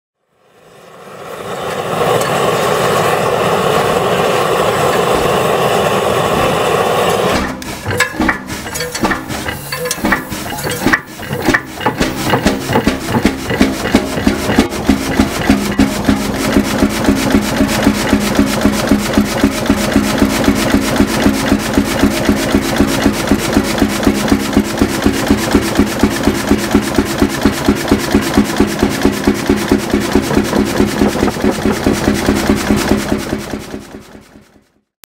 Storebro hot bulb engine
Weighing two and a half tons this hot bulb engine is a beast! It was built in 1936 by Storebro mechanical works in Sweden and installed in a sawmill in Dalarna. The ground really shakes as it operates and unleashes its’ power.  It takes about five minutes before the engine run smoothly.
Recorded at Jäders bruk.
Manufactorer: Storebro mekaniska  verkstad AB
Place of manufactory: Storebro, Sweden
Year of manufactory: 1936
Engine speed: 300 rpm
Engine power: 40 hp
dB: 75